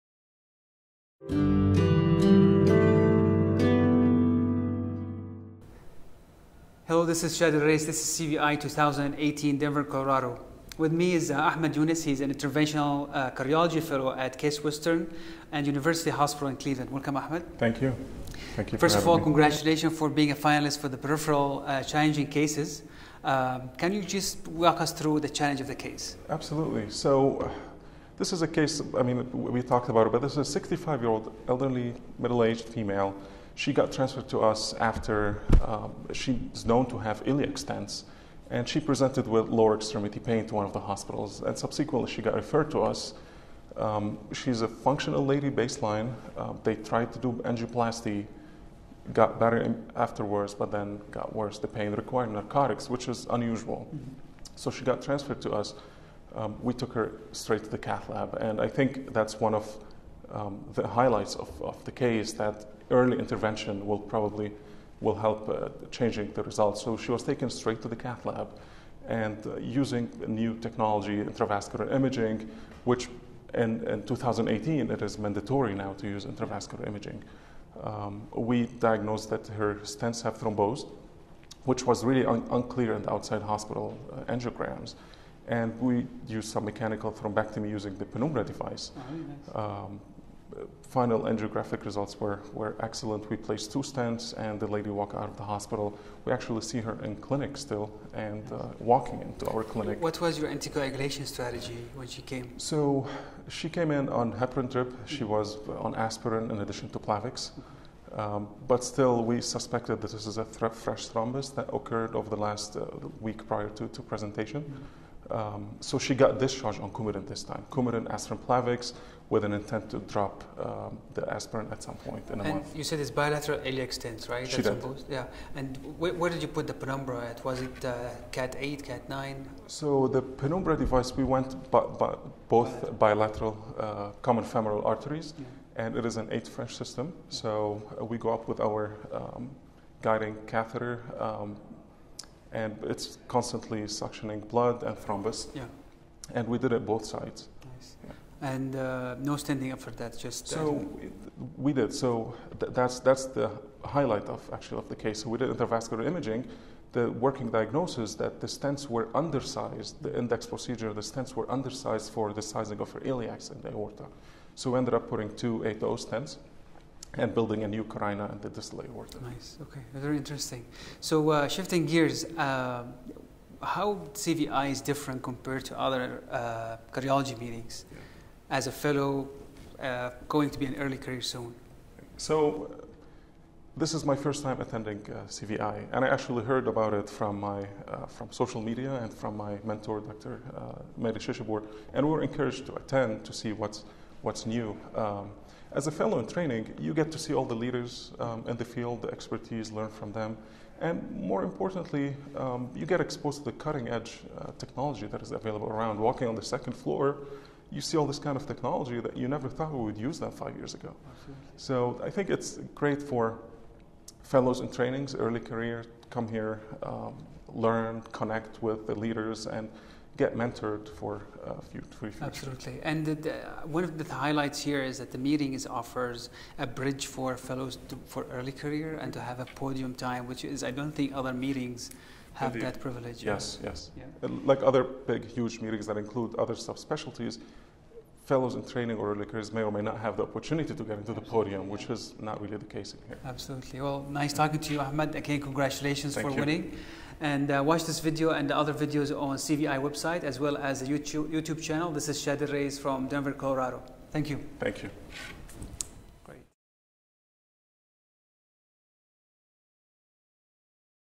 CVI2018 Faculty Interview: Ahmad Younes, MD
CVI2018 was hosted by Cardiovascular Innovations Foundation at the Grand Hyatt Hotel Denver, Colorado, July 26-28, 2018. View information on upcoming meetings at cvinnovations.org